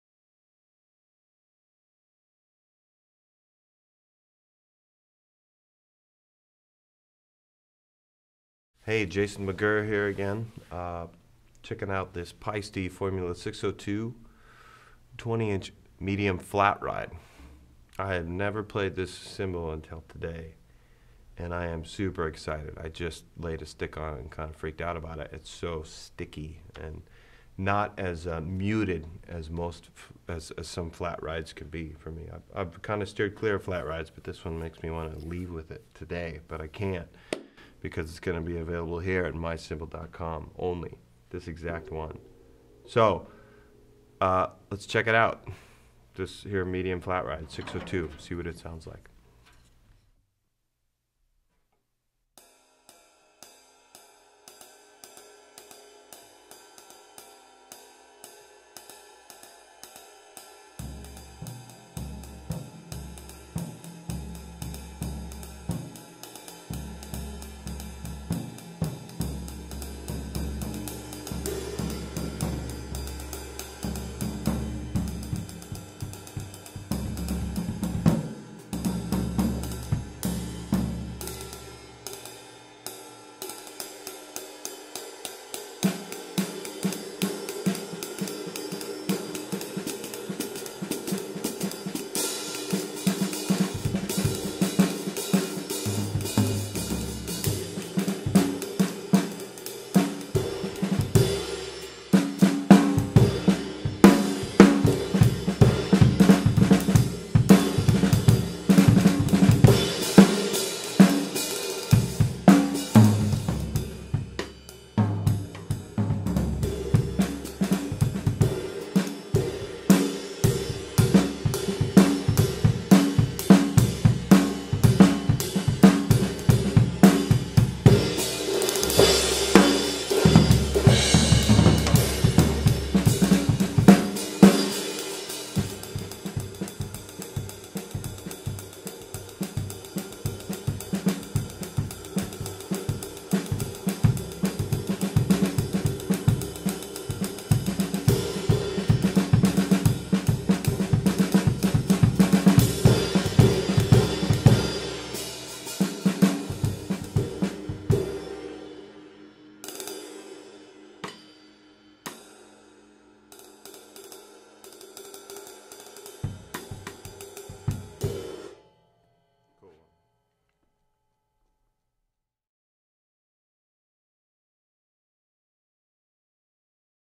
Paiste 20" Formula 602 Medium Flatride Cymbal - Played by Jason McGerr (1042520-1050212E)
*SOLD* Paiste 20" Formula 602 Medium Flat Ride Cymbal available from myCymbal.com. See more cymbals like this

Other gear used in this video:
• Cymbals (From drummer's L to R):  Paiste 14" Formula 602 Sound Edge Hi Hat Cymbals, Paiste 18" Formula 602 Thin Crash Cymbal
• Drum kit:  Craviotto Solid Shell Maple Drum Set 13", 16", 22"
• Snare:  Craviotto Stacked Maple/Poplar/Maple 14" x 5"